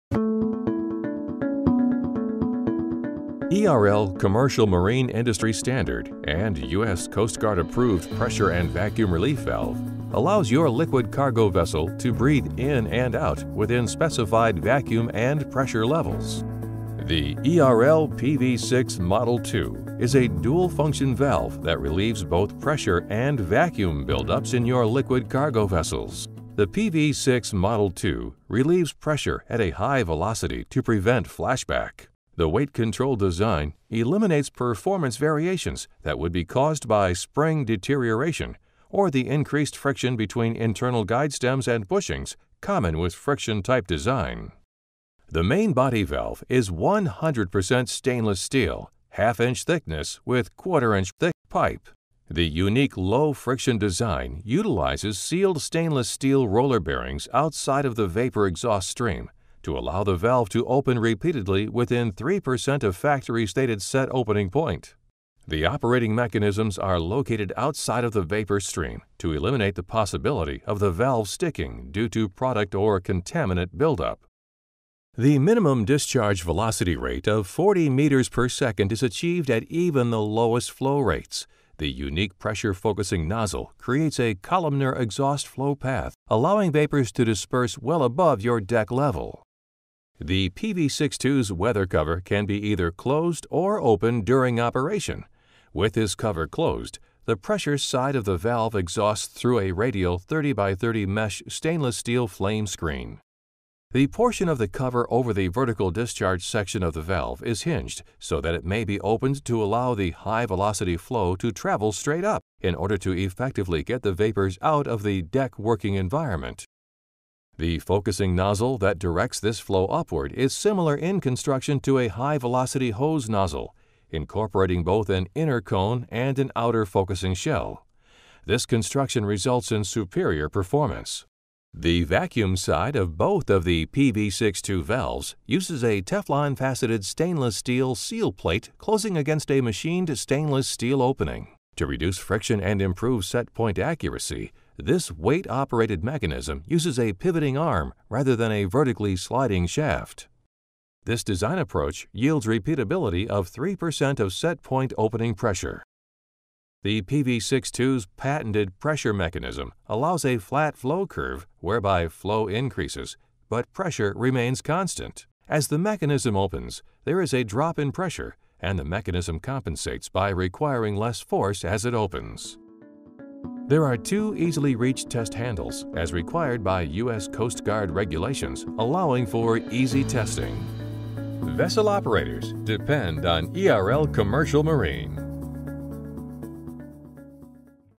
ERL Inc. - Superac PV High Velocity P-V Valve
The ERL Superac 6" High Velocity weight operated P-V Valves operates automatically on both the High Velocity pressure side and the vacuum side of each valve. Mounted on a liquid cargo vessel, this valve opens at a specific design set point to relieve vessel tank pressure to the atmosphere at a High Velocity to prevent flame propagation into the barge.